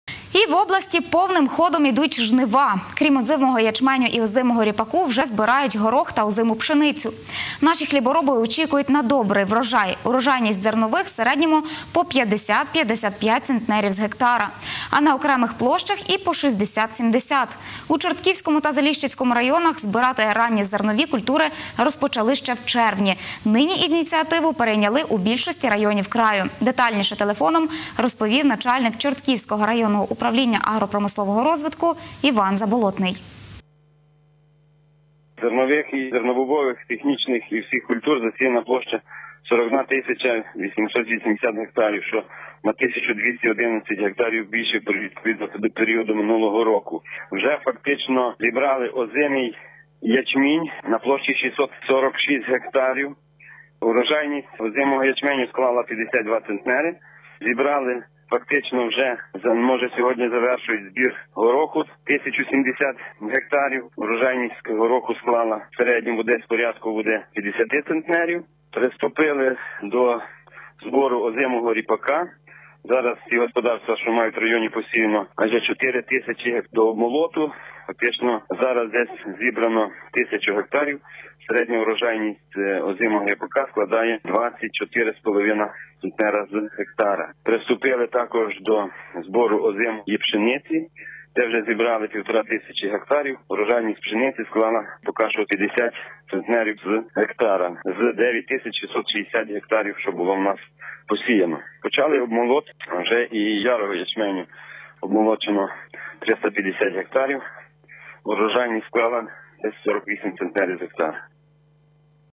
На Тернопіллі повним ходом йдуть жнива
В області повним ходом йдуть жнива. Крім озимого ячменю і озимого ріпаку вже збирають горох та озиму пшеницю. Наші хлібороби очікують на добрий врожай. Урожайність зернових в середньому по 50-55 центнерів з гектара, а на окремих площах – і по 60-70.